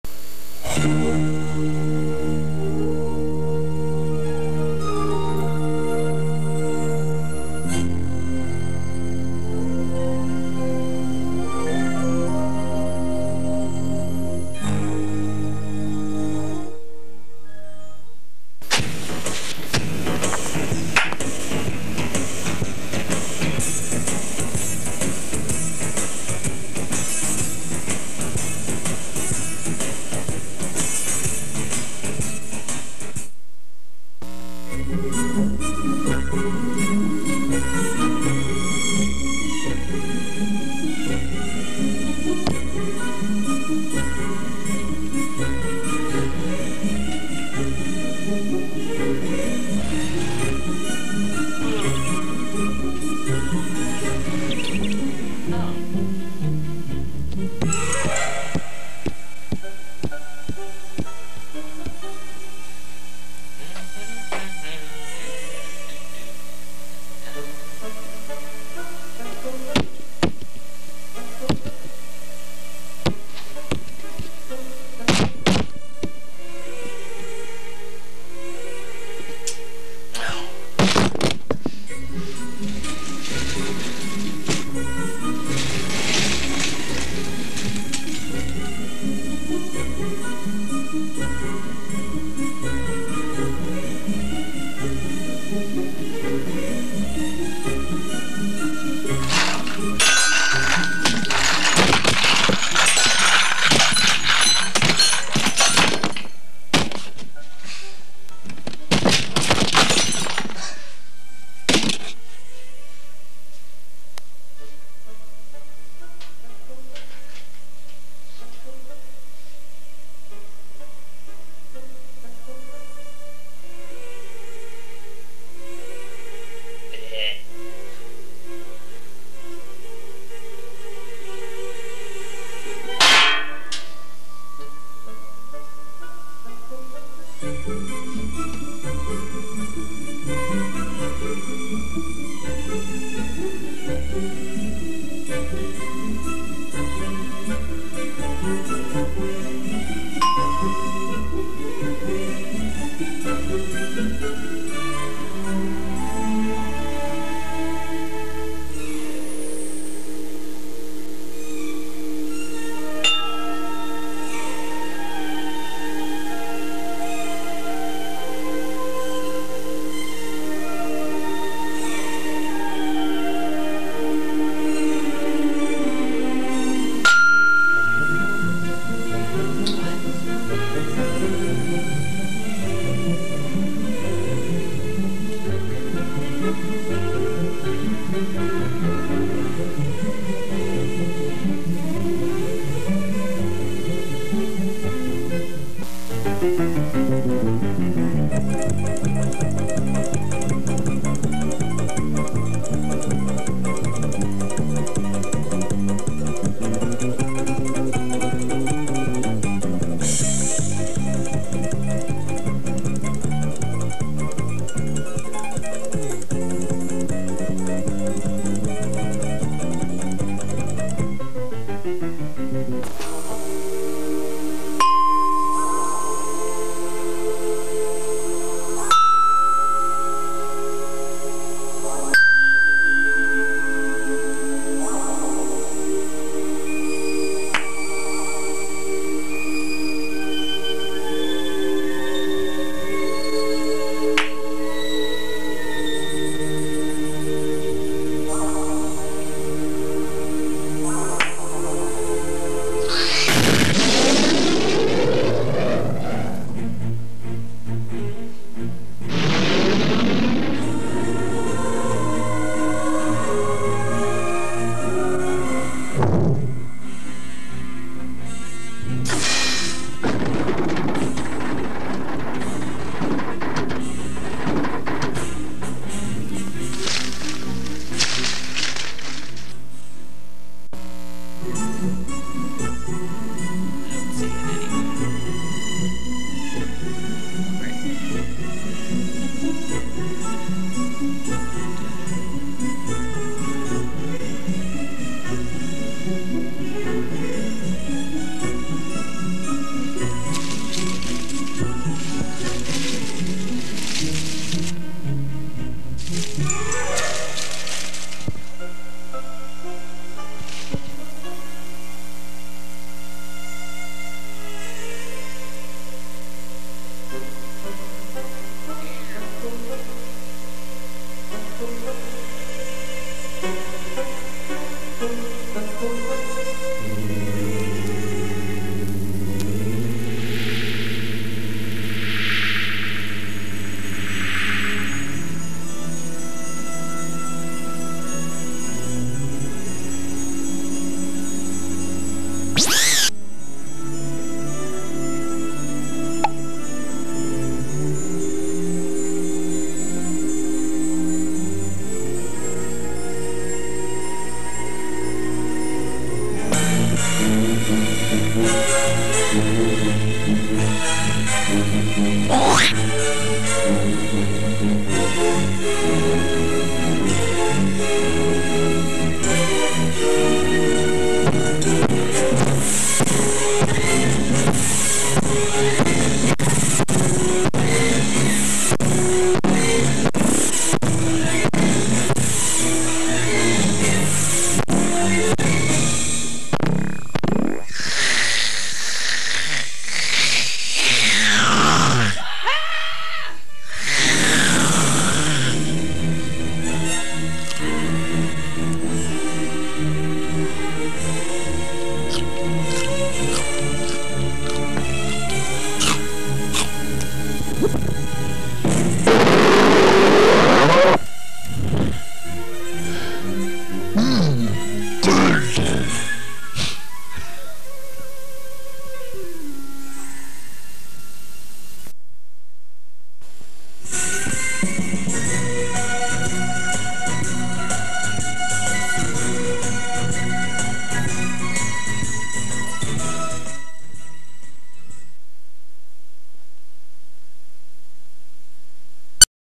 Runaway Croquet
This is vintage animation production done on 16mm film without the aid of computers. Transferred to VHS years ago. Looks much better on the original film. I produced this around 1981 using a Bolex camera. Remember when you had to wait to get the film back from developing to see how it turned out? I will be putting more of our old productions up here as I transfer the film to digital format.